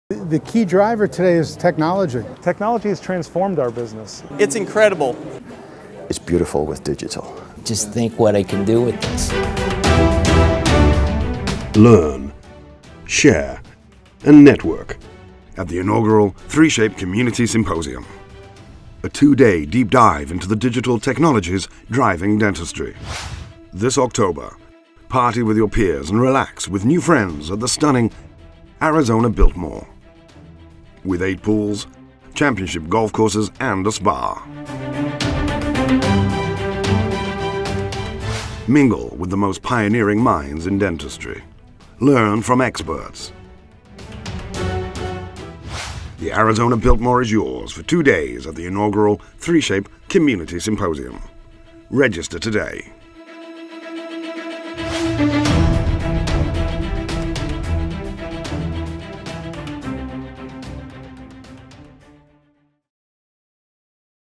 3Shape Community Symposium Overview
Join 3Shape at the 3Shape Community Symposium for a deep dive into the digital technologies and workflows that are driving dentistry, with presentations from some of the most pioneering minds in the industry. To learn more and register, please go